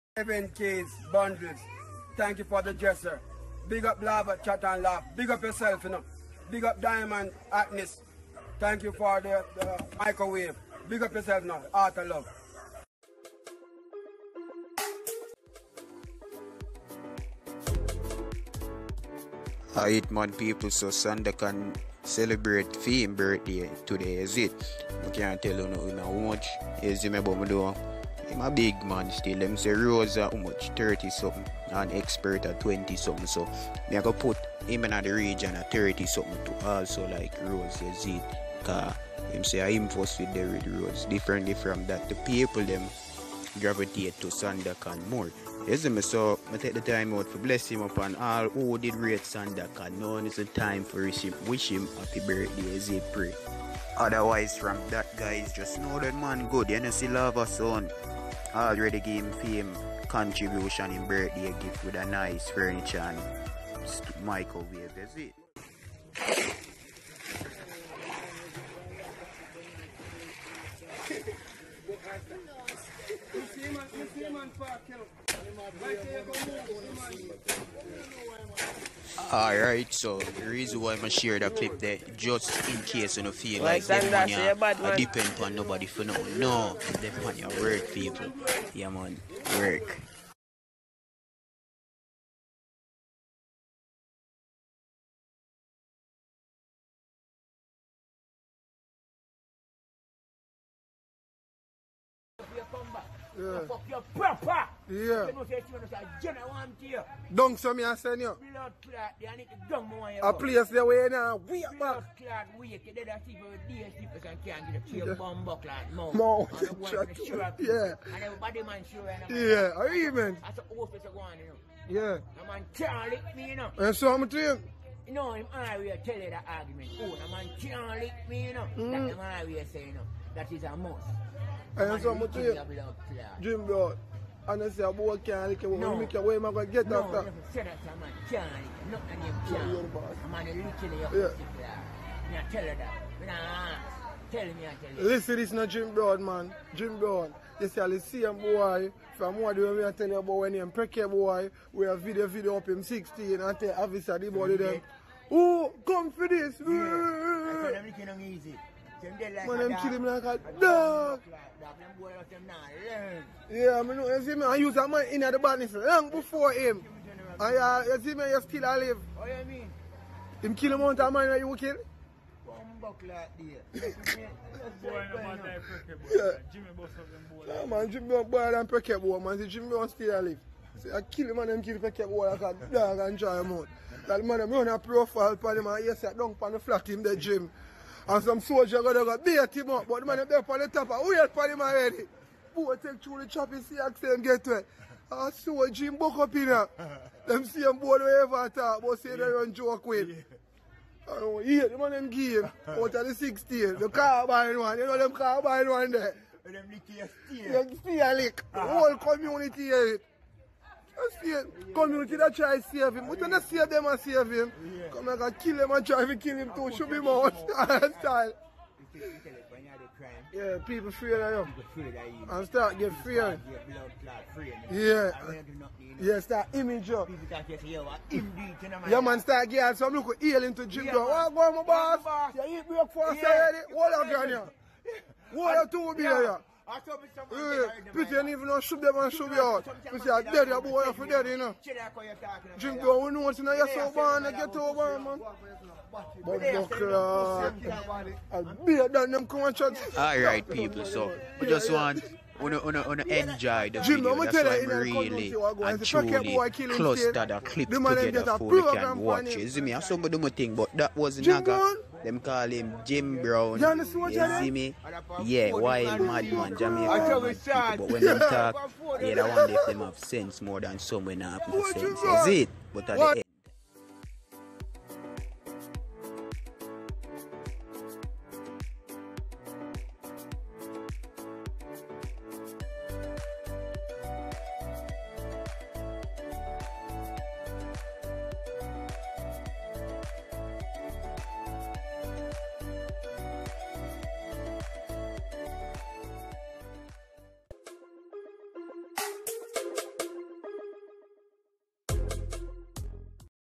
Sandocan Get More Things On His Birthday From Lava Sound *Furniture & Microwave* | Nagga Don
Sandocan birthday
Rose videos
Jamaican Mix Up & Fun Videos